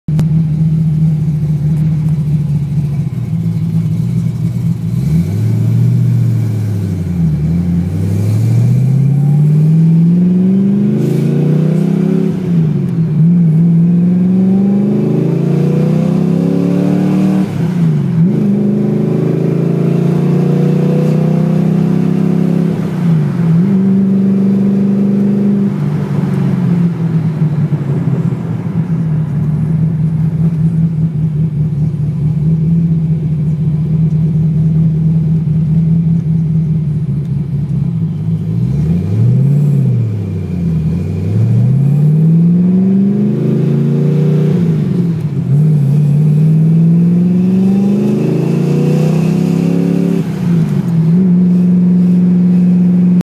Dart 347/ Vortech YSi 3" exhaust in car
In car iPhone video of Flowmaster 17389 Delta Force 3" exhaust system